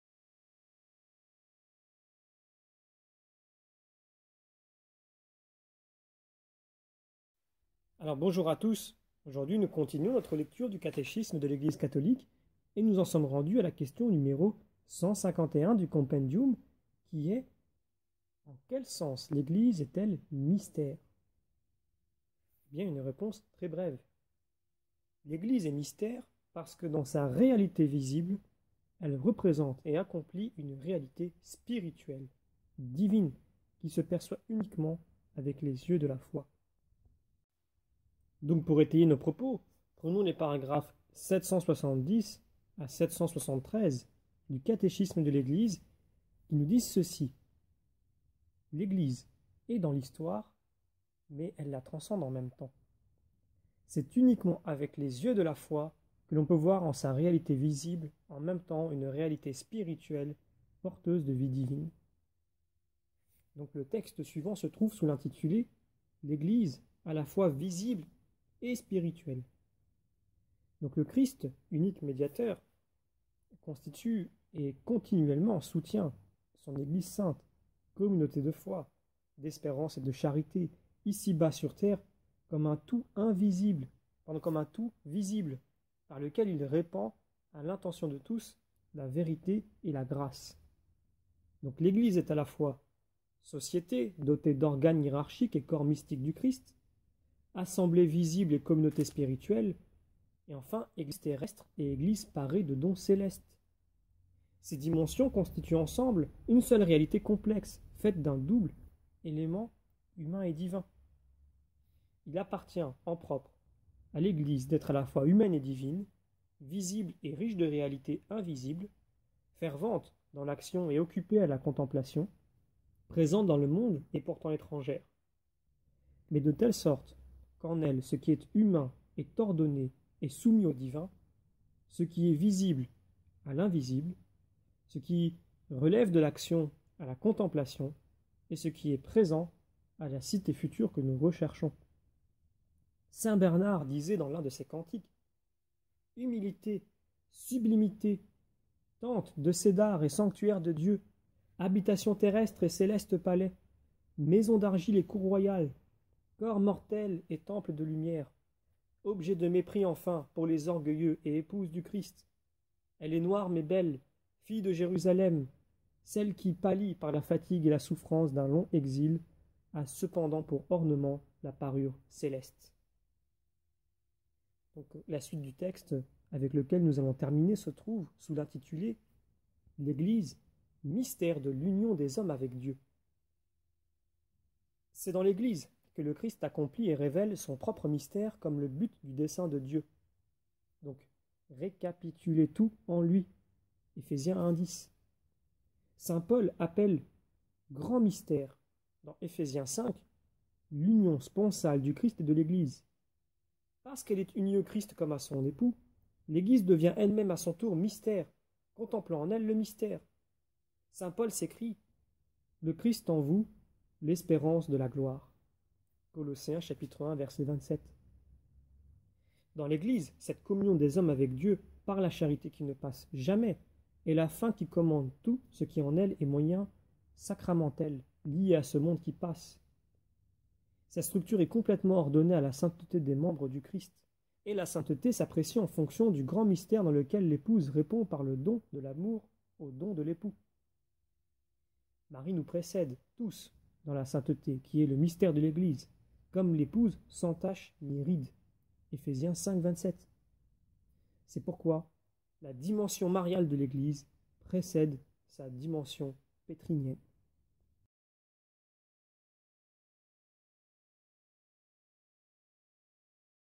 En quel sens l’Église est-elle Mystère ? Compendium du Catéchisme de l’Église Catholique
En quel sens l’Église est-elle Mystère ? Question n°151 du Compendium du Catéchisme de l’Église Catholique.